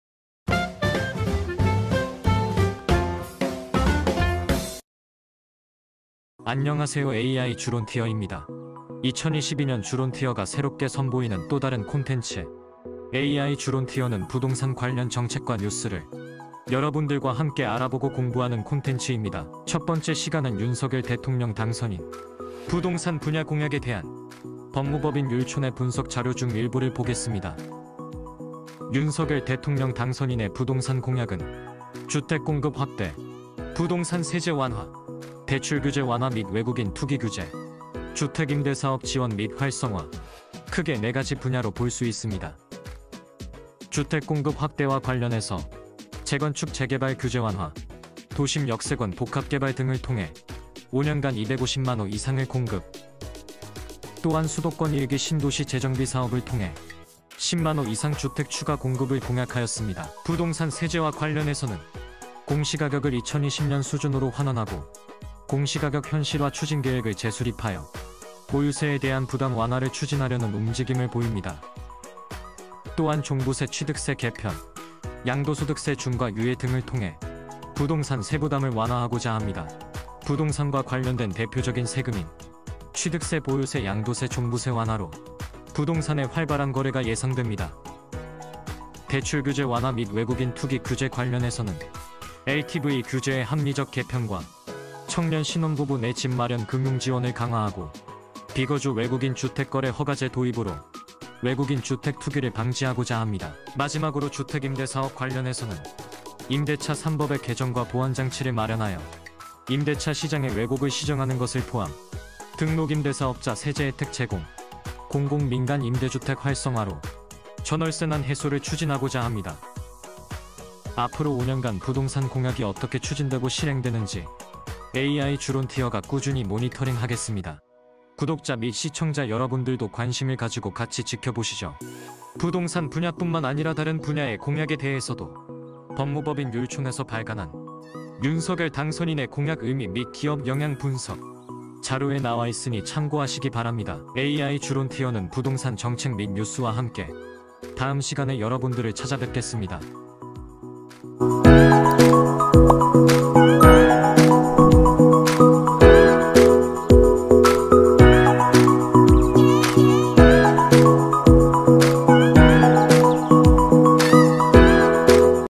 [AI 주론티어] 부동산 정책 공약 체크포인트 (feat. 윤석열 대통령 당선인)
2022년 주론티어가 선보이는

또 다른 New 콘텐츠는

"AI 주론티어" 입니다

AI 주론티어는 부동산 정책 및 뉴스 등을

구독자 및 시청자 여러분들께 소개하는

콘텐츠입니다.

AI 주론티어 첫 번째 시간은

윤석열 대통령 당선인의

부동산 정책 공약을 체크해봅니다!